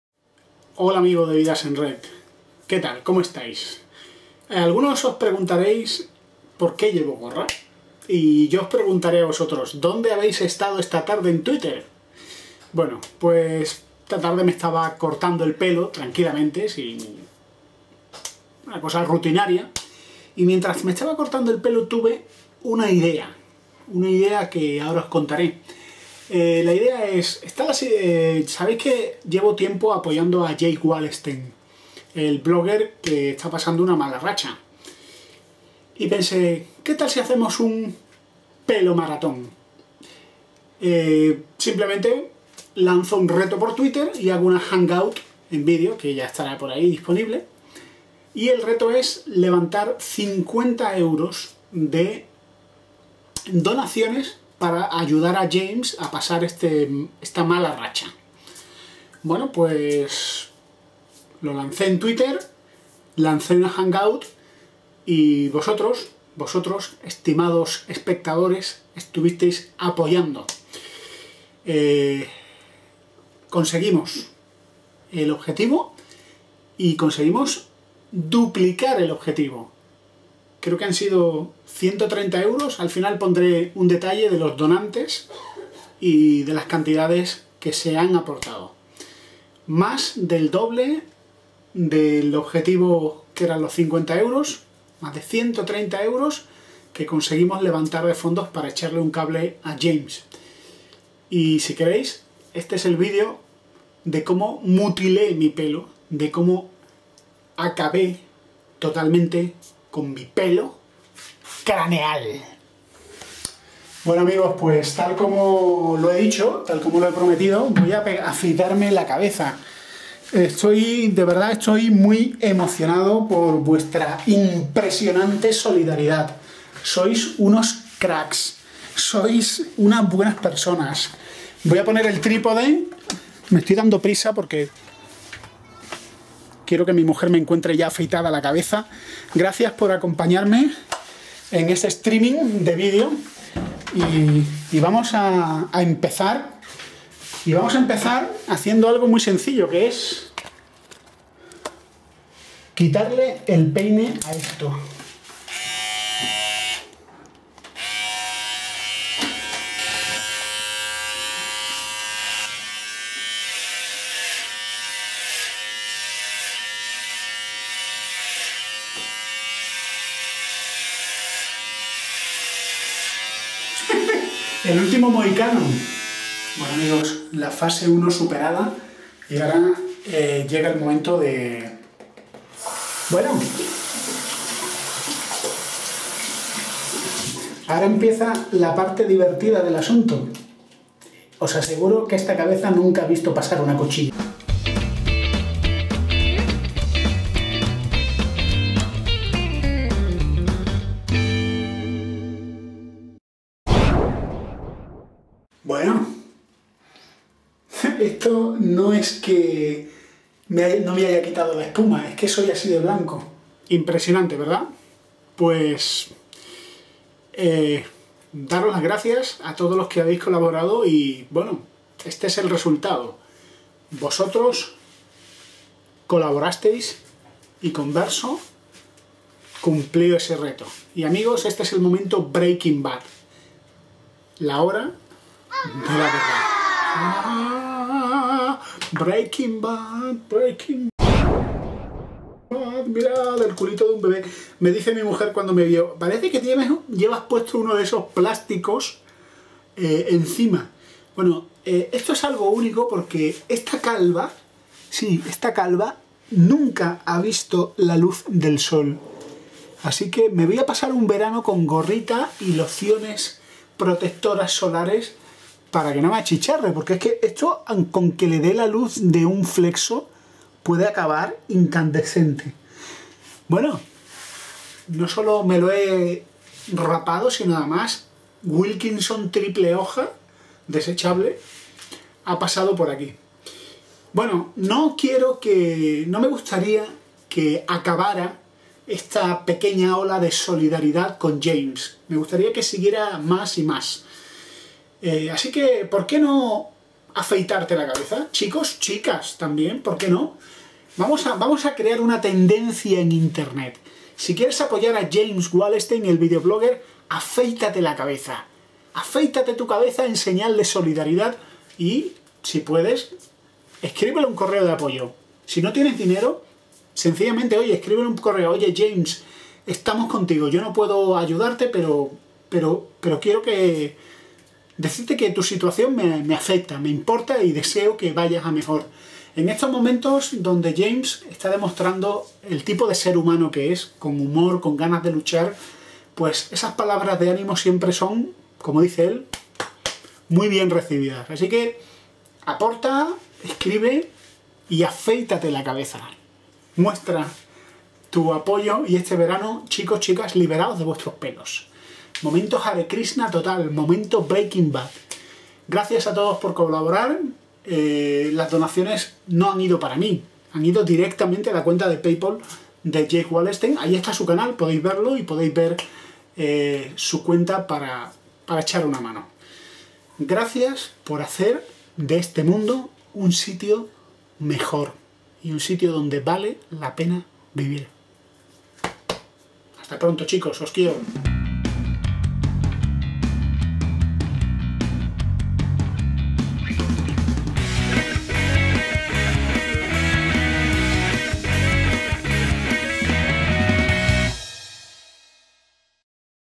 Apoyando a James Wallestein con un reto
Como parte de la campaña de apoyo al vlogger James Wallenstein, aquí está el cumplimiento del reto que presenté en Twitter: afeitarme la cabeza en caso de alcanzar un mínimo de donaciones para James.